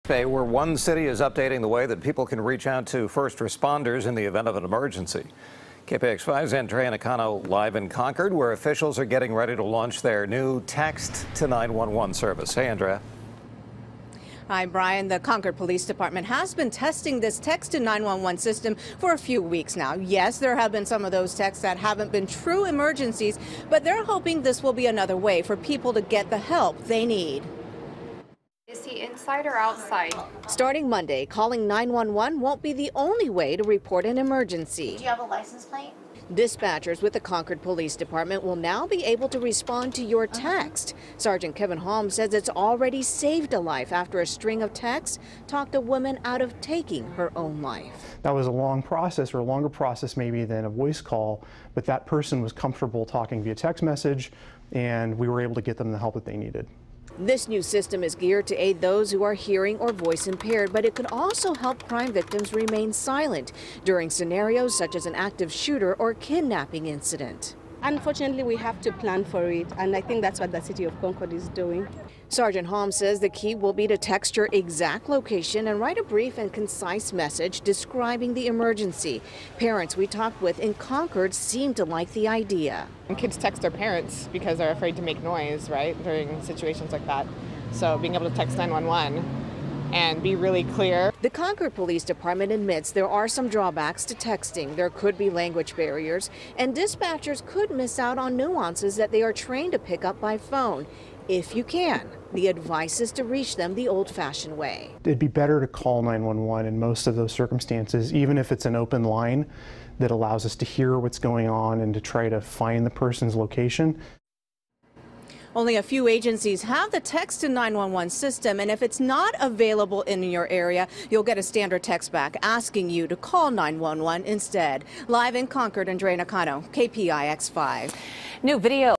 CONCORD: Officials launching new texting 911 system
Officials launching new texting 911 system